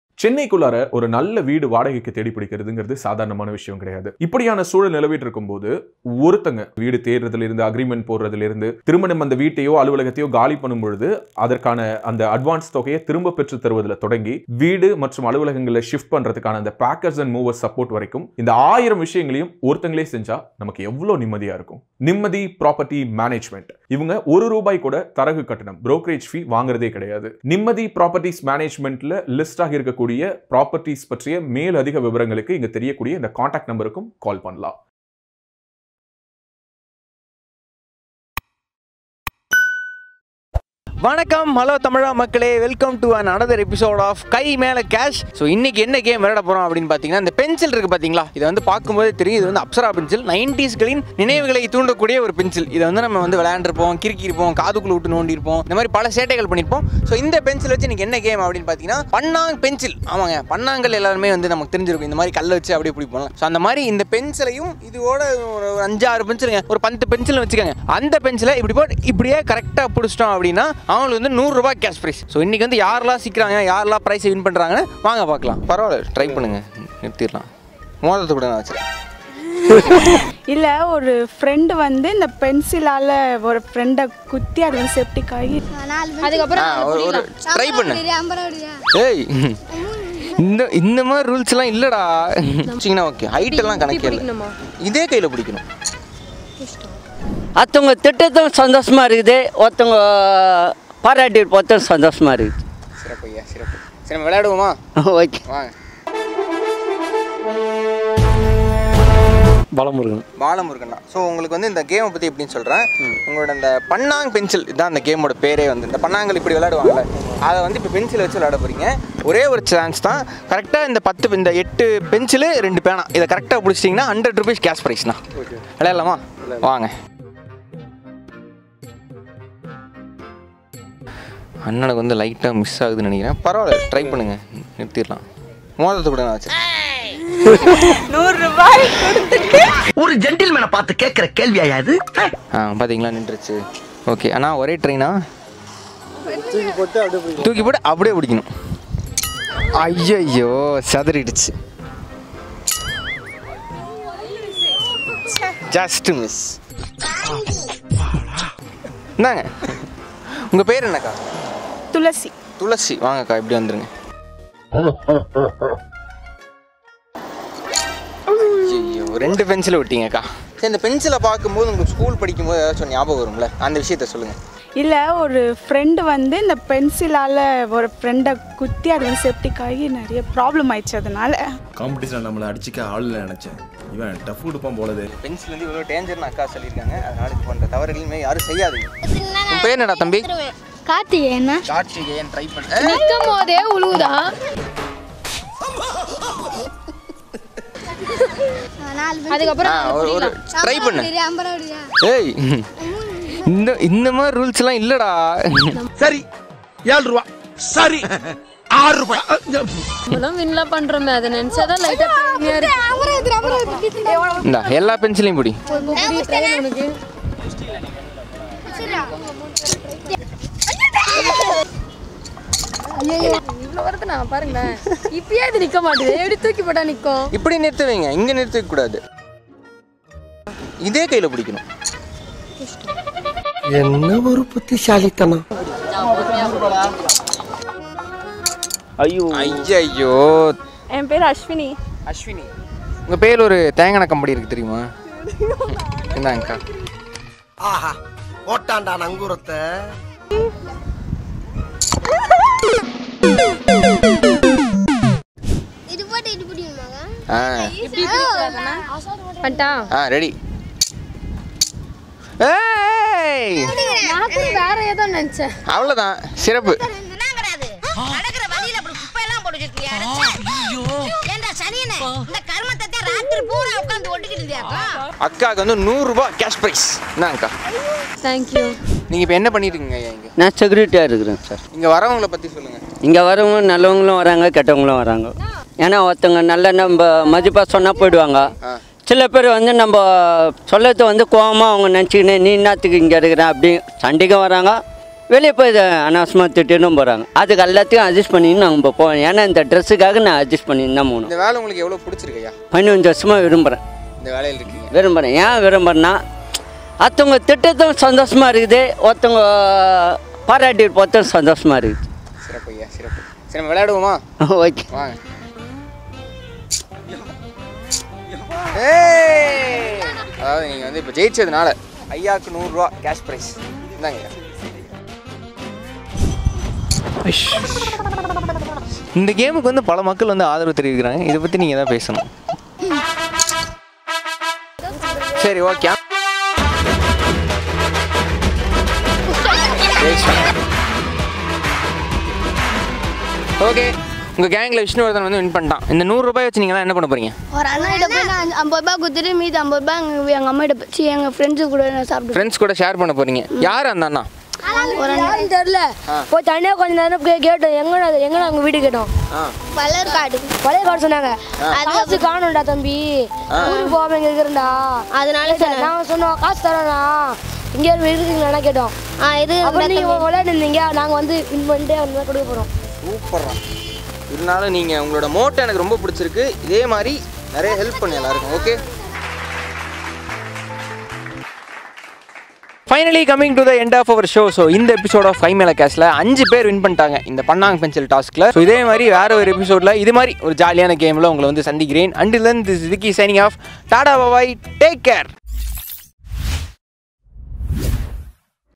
Pencil Catching Challenge - பென்சில Friend காதுல குத்திட்டேன்😱 | Kai Mela Cash | Hello Tamizha
Kai Mela Cash is a new fun game, meeting random people and challenging with different games. Winner of the game will be getting exciting cash price.

CEO
Rajavel Nagarajan

© Pesu Tamizha Pesu Private Limited